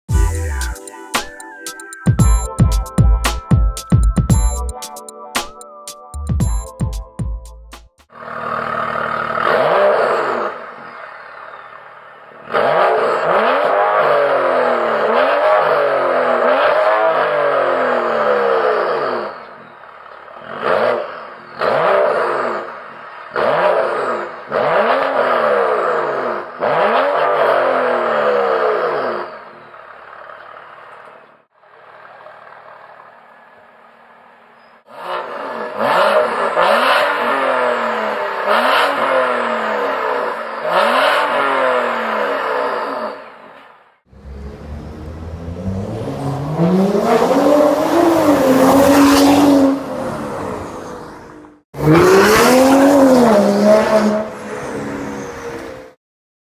BMW Active Hybrid 3/ 335i with Mach5 valvetronic exhaust
-made in T304 stainless steel
-Maximum air flow design for performance
-3 valve mode: auto, open, close
-Best quality precision machining & Welding & Craftsmanship
-Best product to reinforce your vehicles and a true attention stealer